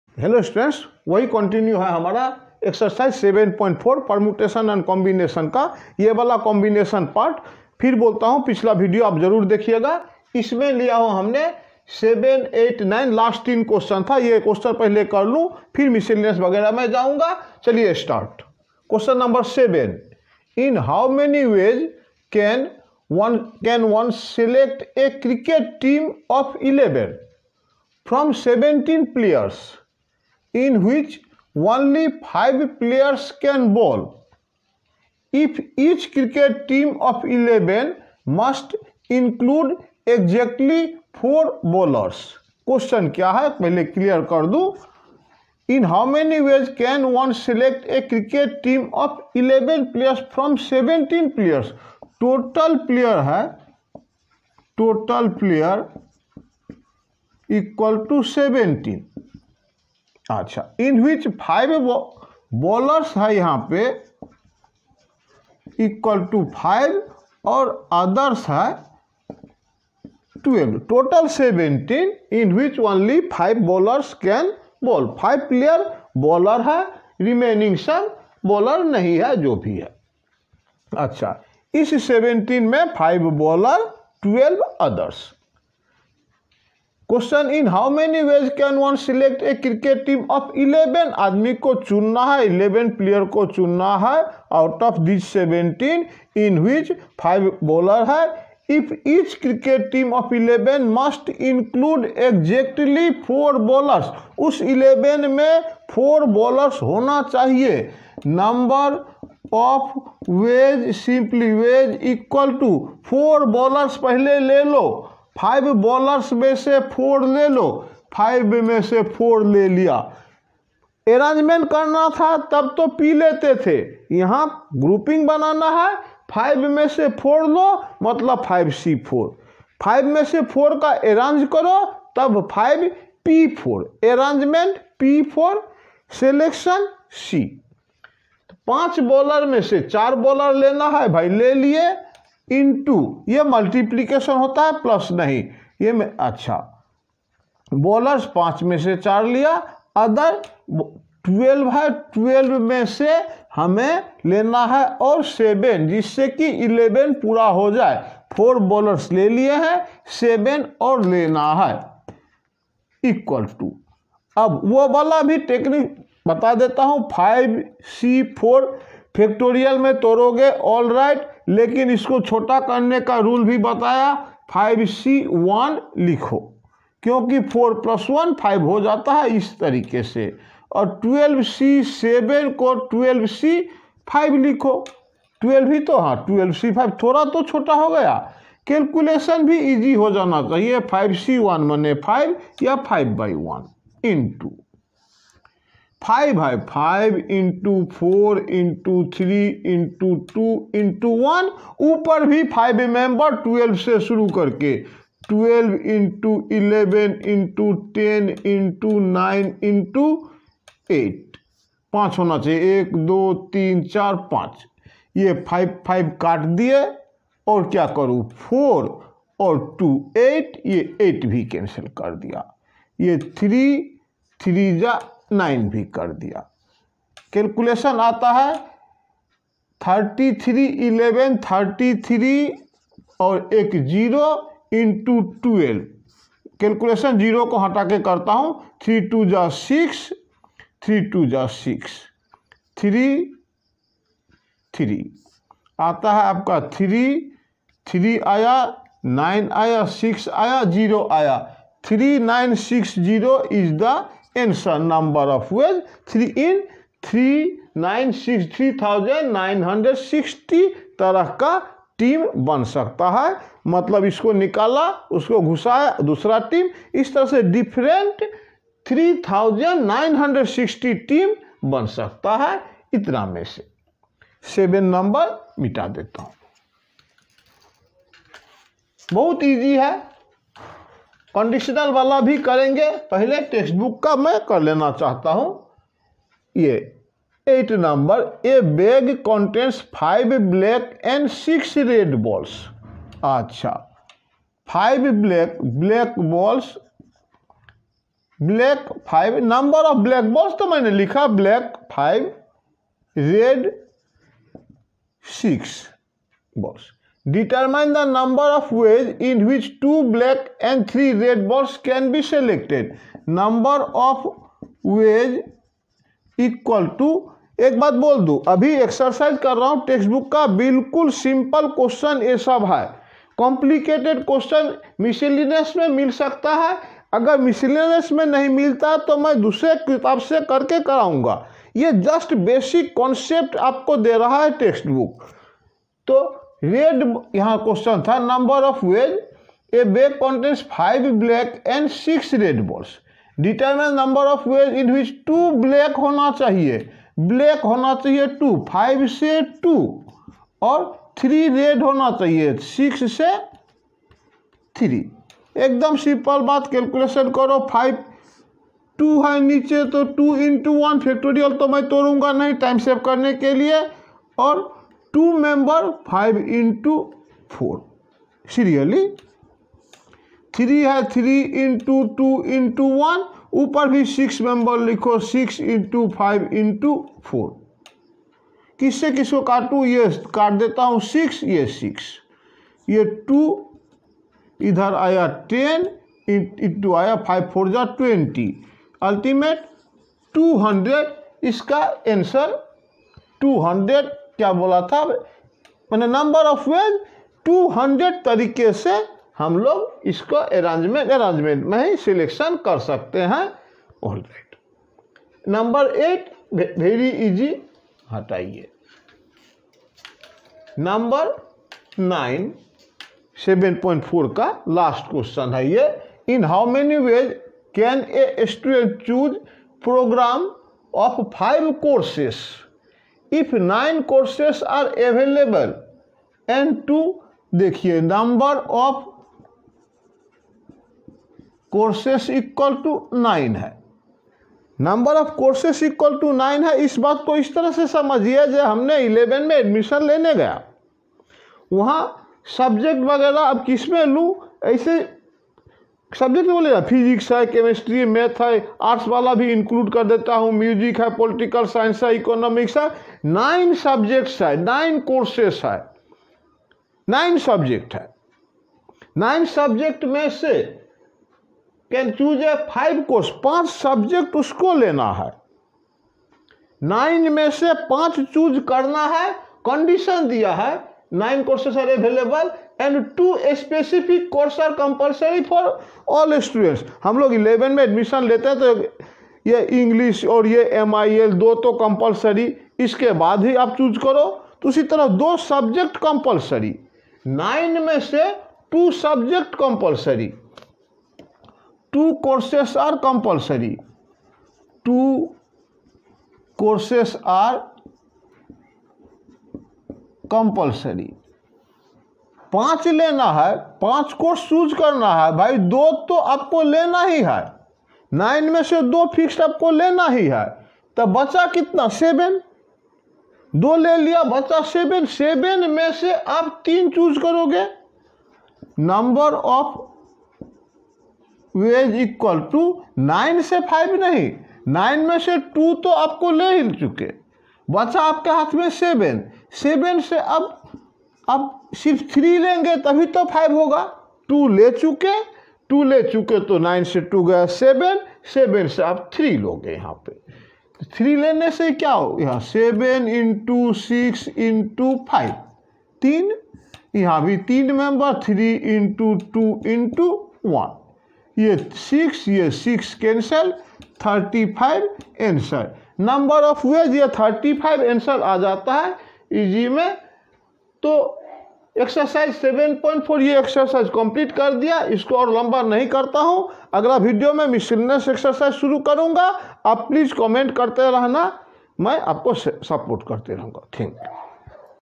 permutations and combinations for class 11,part 10(ex.7.4, q/n.7, 8 & 9)
In this video I have explained the solution for question number 7,8 & 9 of ncert exercise 7.4 for class 11.